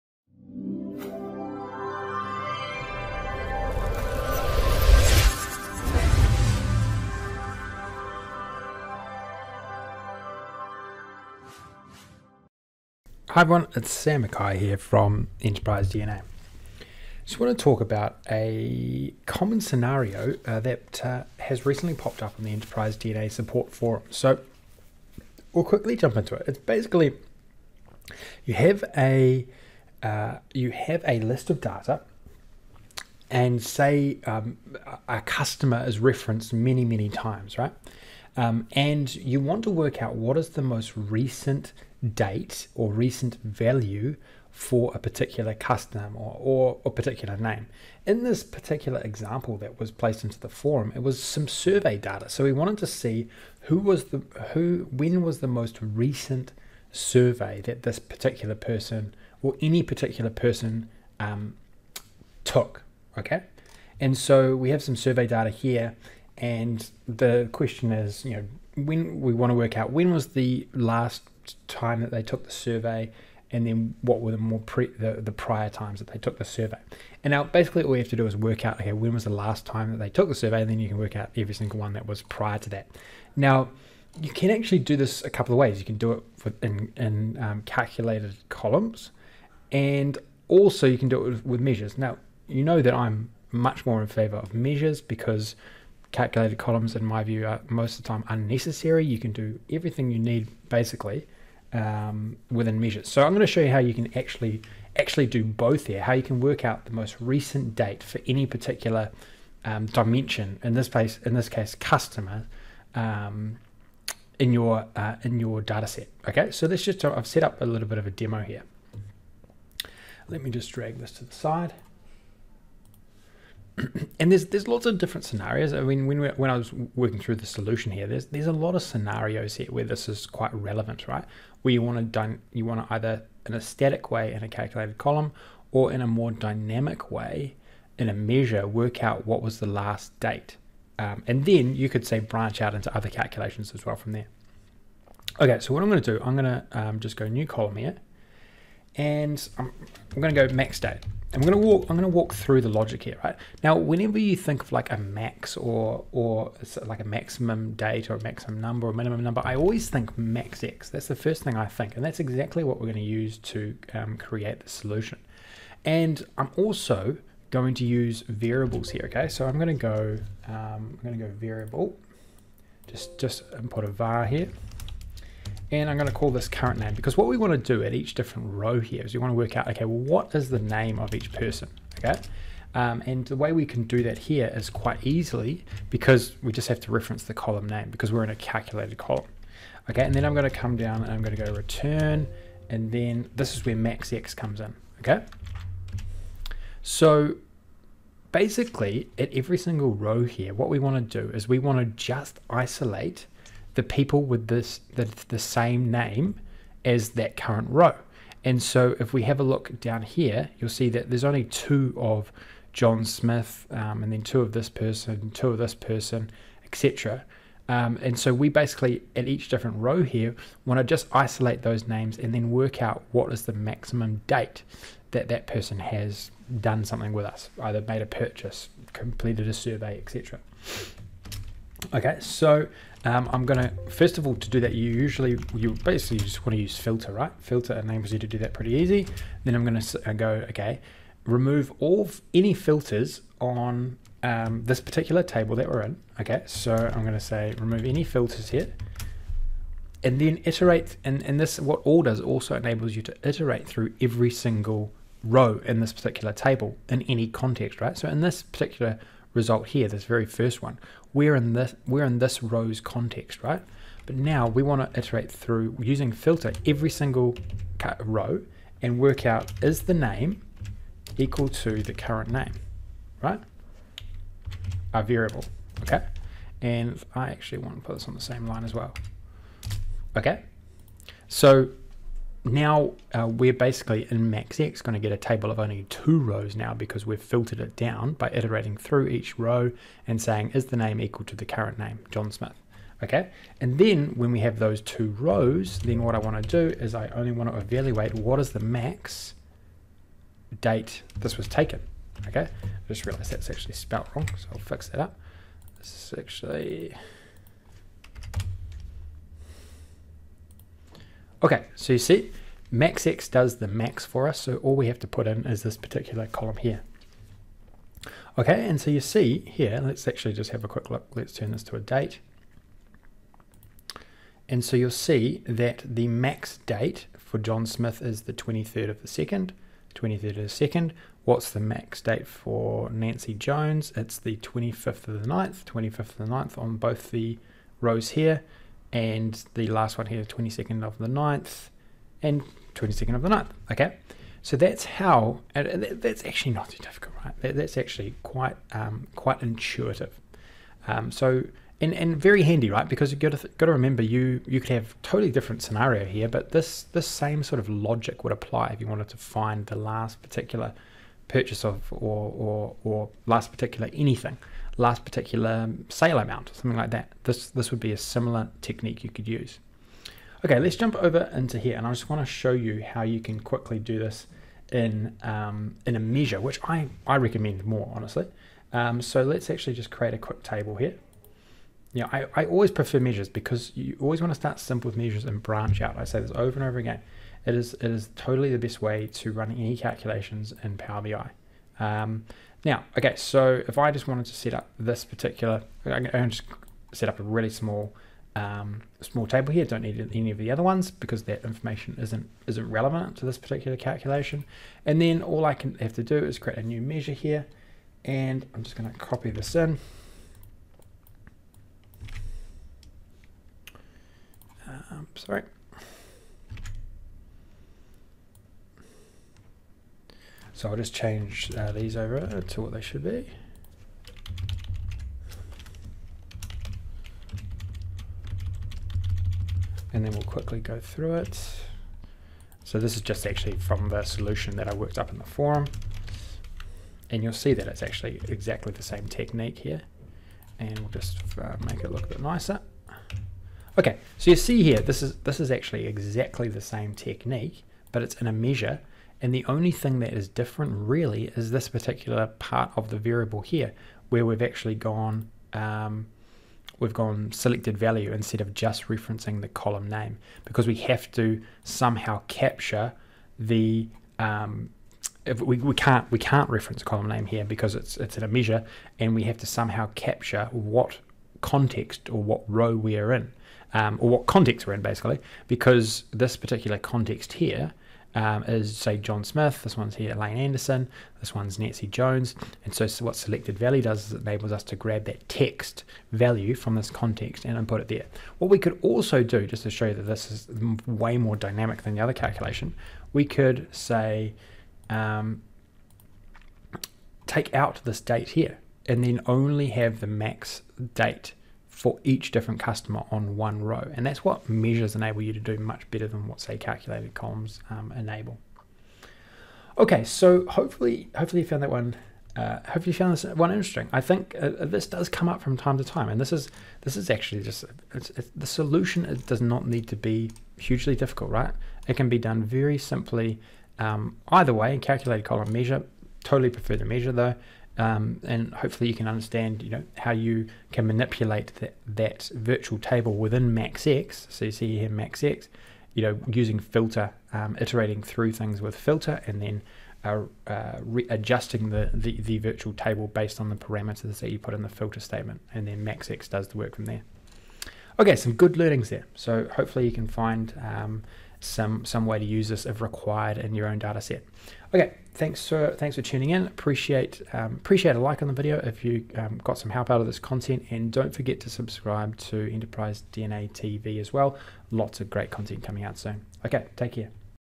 Calculate The Last Or Max Value Within A Table Of Data Using DAX In Power BI
In this tutorial, we review how you can work out the last value from a particular dimension in a table in Power BI.

This is quite a unique insight but a relevant one from many pieces of analysis that you might require in Power BI.

We work through examples of the MAXX and FILTER functions. These are two important functions in the DAX language that can be used in many situations to extract very interesting insights from your data.

Good luck.

Sam

Video Details
2:27 - using calculated columns
6:13 - using a measure

Last Purchase Date In Power BI: When Did Your Customers Make Their Last Purchase?

For more calculate recent value support queries to review see here….